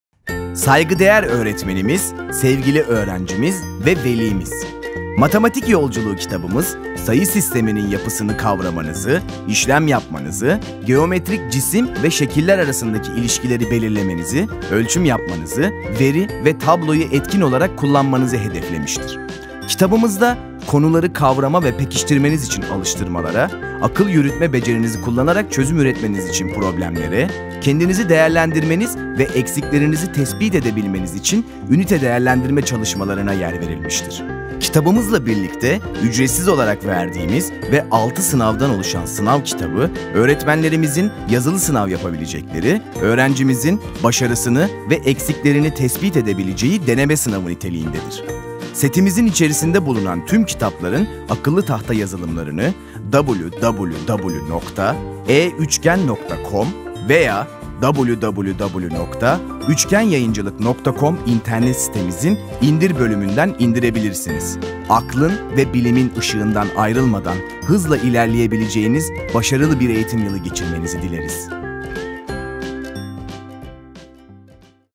2. Sınıf - Matematik Yolculuğu Tanıtım Videosu
Saygıdeğer Öğretmenimiz, Sevgili Öğrencimiz ve Velimiz,

   Matematik Yolculuğu kitabımız; sayı sisteminin yapısını kavramanızı, işlem yapmanızı, geometrik cisim ve şekiller arasındaki ilişkileri belirlemenizi, ölçüm yapmanızı, veri ve tabloyu etkin olarak kullanmanızı hedeflemiştir.

   Kitabımızda
        Konuları kavrama ve pekiştirmeniz için alıştırmalara,
        Akıl yürütme becerinizi kullanarak çözüm üretmeniz için problemlere,
        Kendinizi değerlendirmeniz ve eksiklerinizi tespit edebilmeniz için Ünite Değerlendirme çalışmalarına
yer verilmiştir.

   Kitabımız ile birlikte ücretsiz olarak verdiğimiz ve altı sınavdan oluşan Sınav Kitabı, öğretmenlerimizin yazılı sınav yapabilecekleri, öğrencimizin başarısını ve ilgili bölüm ile ilgili eksiklerini tespit edebileceği deneme sınavı niteliğindedir.

Setimizin içerisinde bulunan tüm kitapların akıllı tahta yazılımlarını veya İnternet sitemizin “indir” bölümünden indirebilirsiniz.

   Aklın ve bilimin ışığından ayrılmadan hızla ilerleyebileceğiniz başarılı bir eğitim yılı geçirmenizi dileriz."